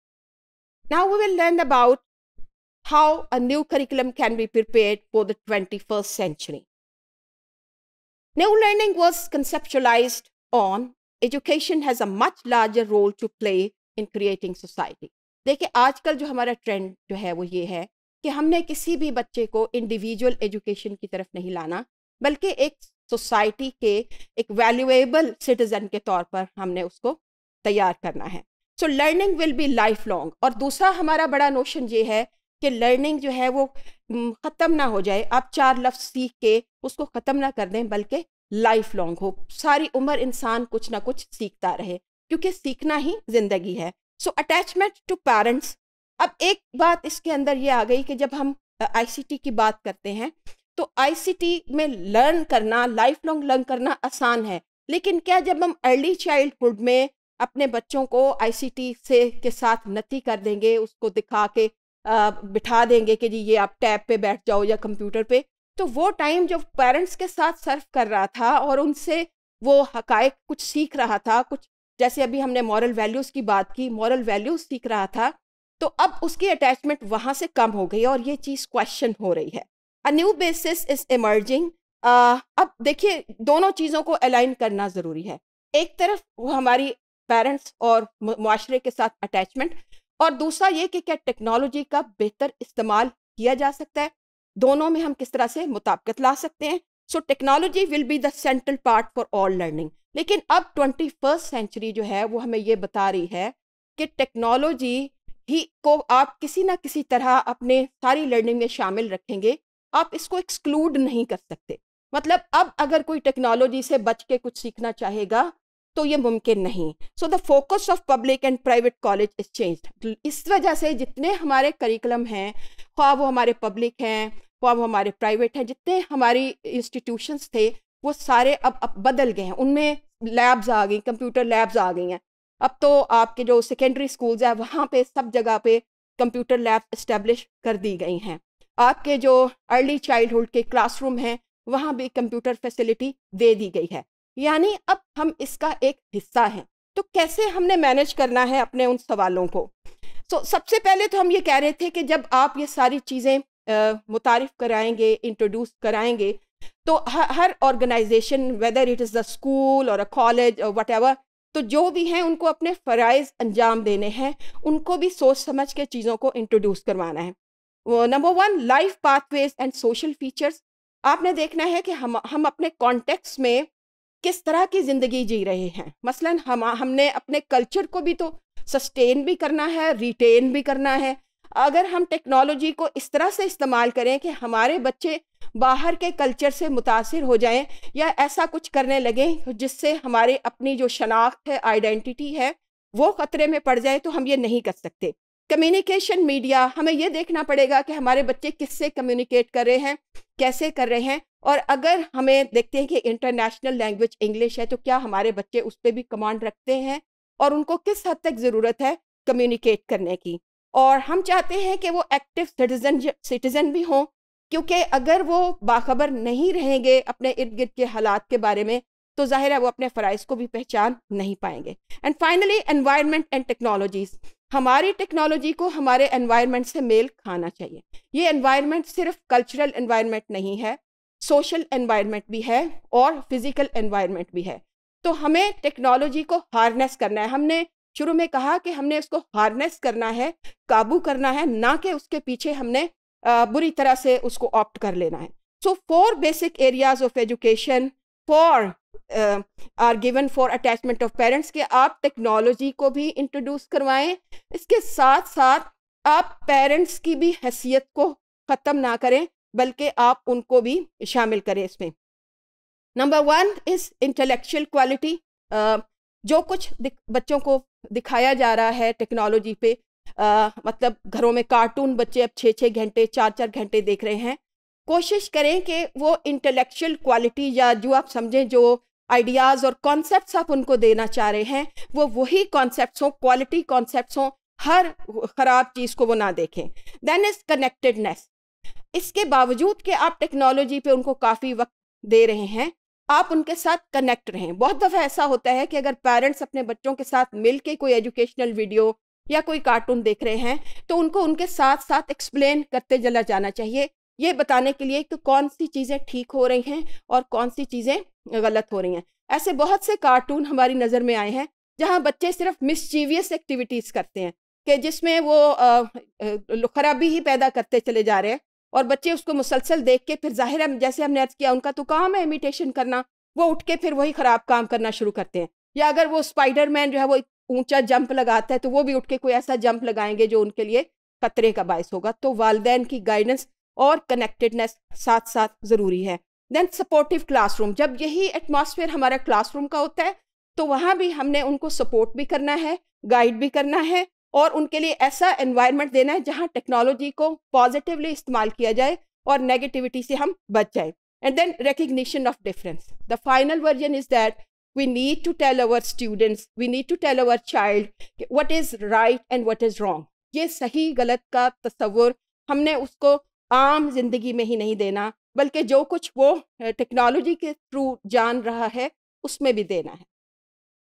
New Curriculum for Twenty-first Century | Trends & Issues in Early Childhood Edu | ECE402_Topic194
Topic194: New Curriculum for the Twenty-first Century,
By Dr. Afifa Khanam